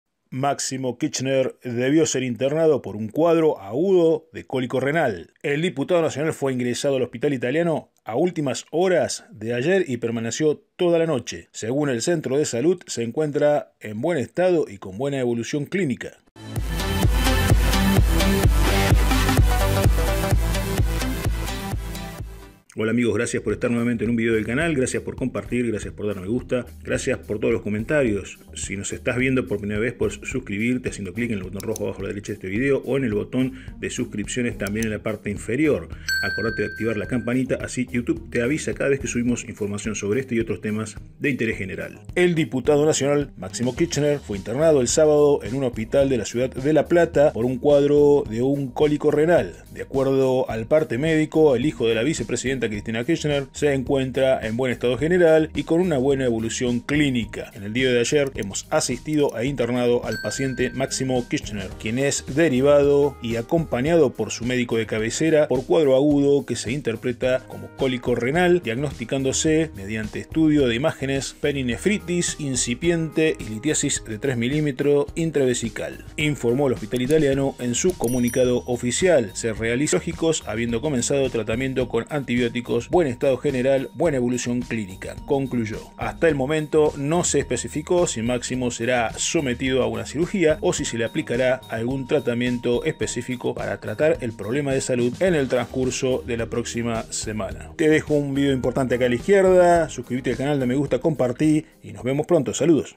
💥💥NOTICIAS de ÚLTIMO Momento💥Internaron a MAXIMO KIRCHNER...
En estas noticias de último moento te contamos que el Hijo de Cristina Kirchner Fue internado ayer en el Hospital Italiano de La Plata.